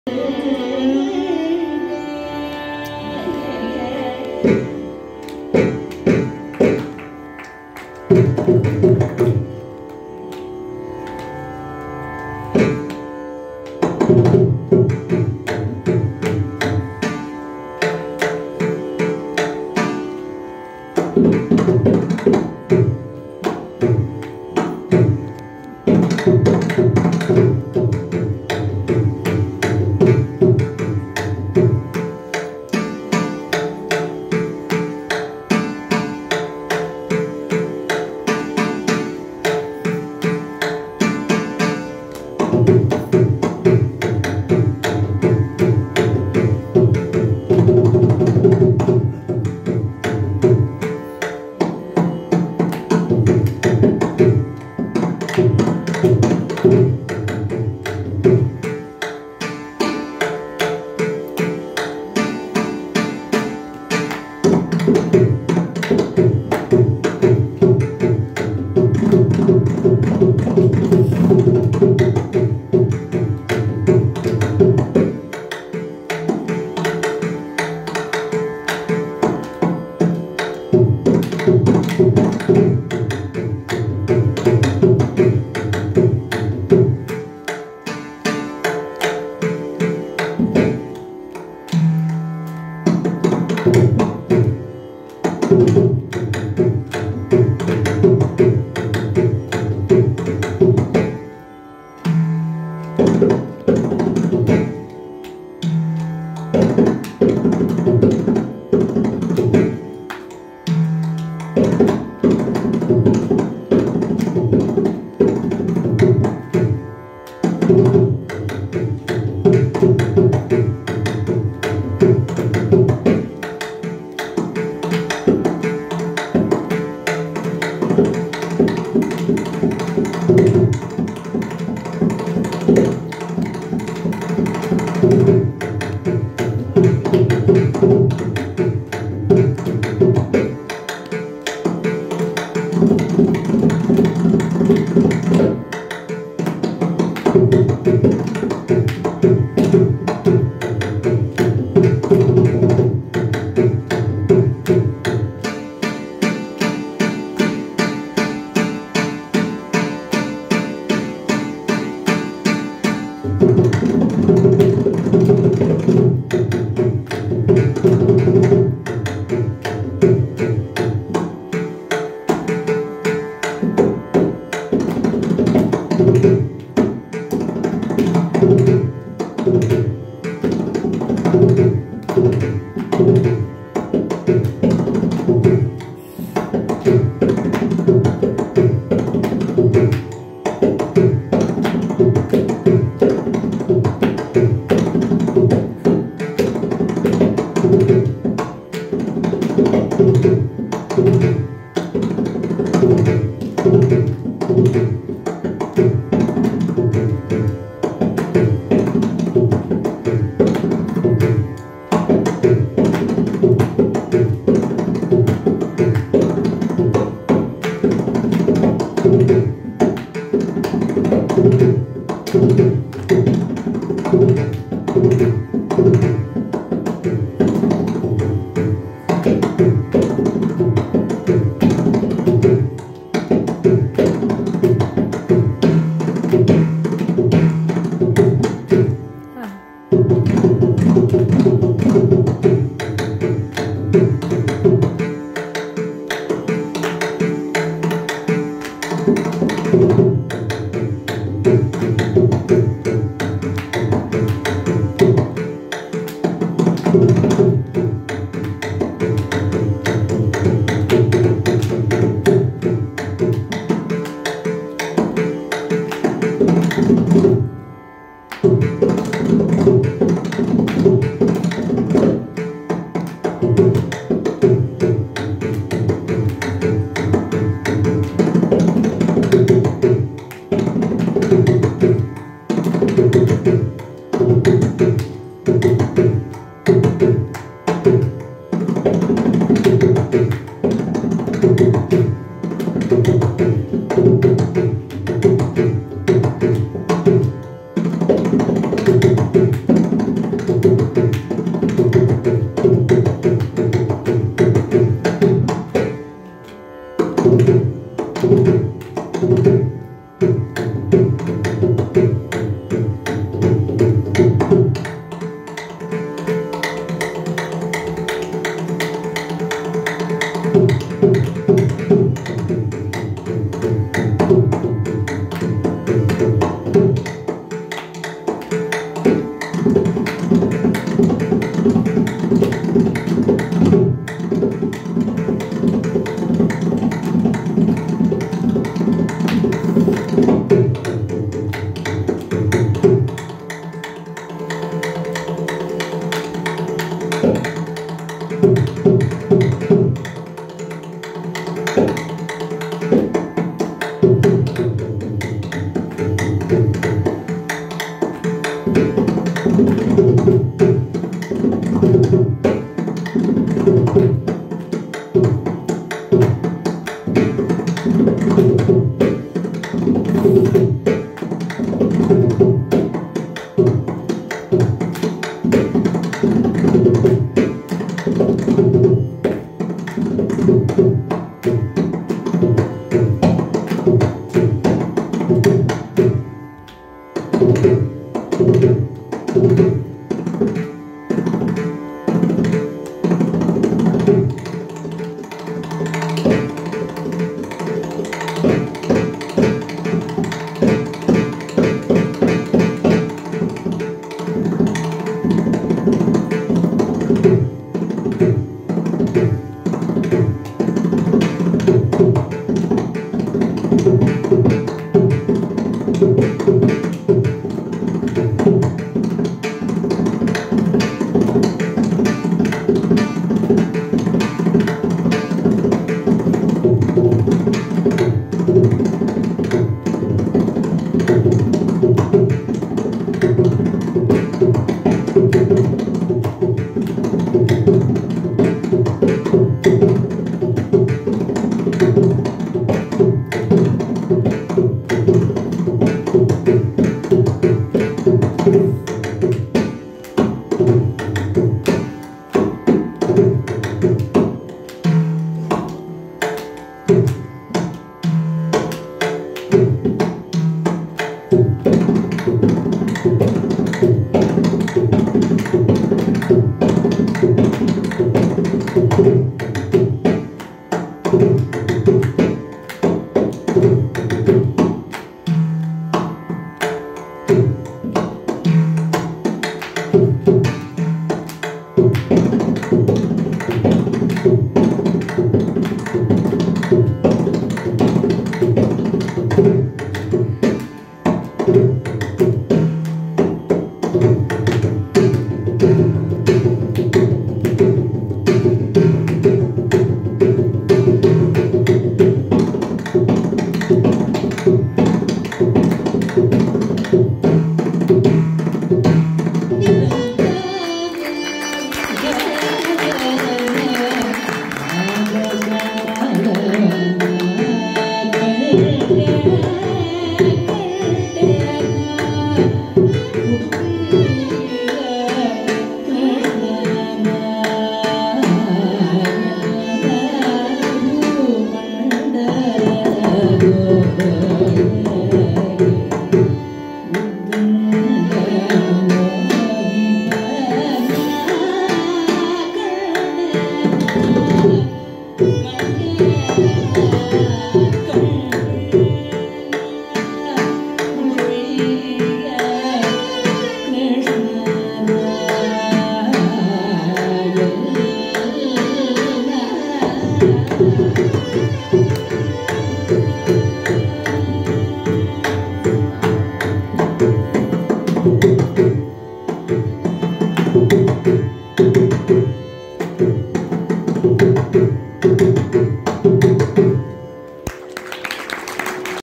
Thani Avartanam in Mishra Chapu Taalam
Vocal - Vid Niranjan Dindodi
Violin - Vid Pradesh Achar
Mridangam - Sunaada Krishna Amai

Excerpt from a live concert held at The Indian Institute of World Culture,Bengaluru on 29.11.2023.